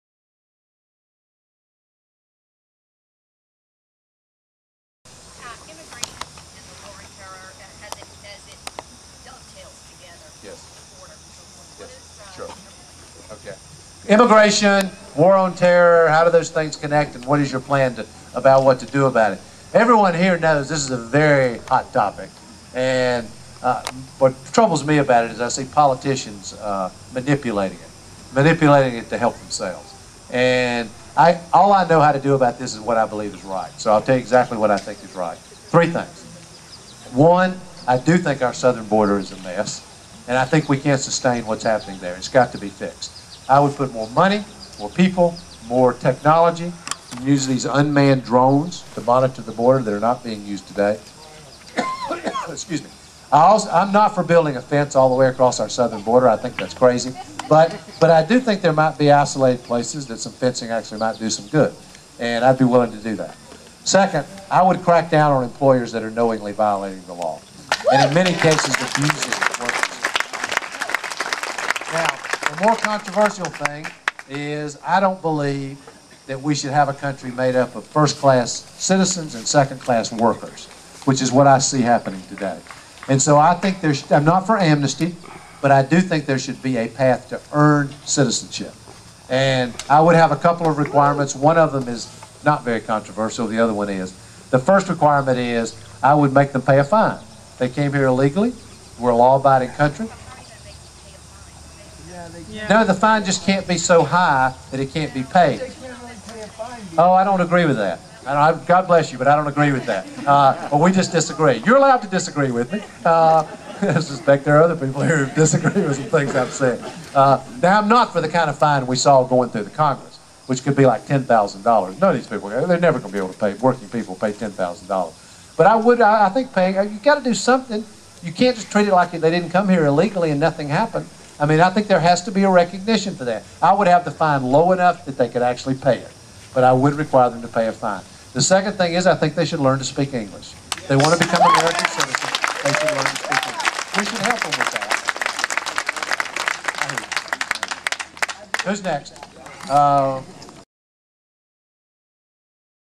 John Edwards Town Meeting
What would you do about Illegal Immigration?

BelowBoston.com was at a town meeting with John Edwards in Londonderry on Sunday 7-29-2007.  Another great event put on by the Edwards Campaign. Perfect weather, Location and John answered a lot of question from the voters.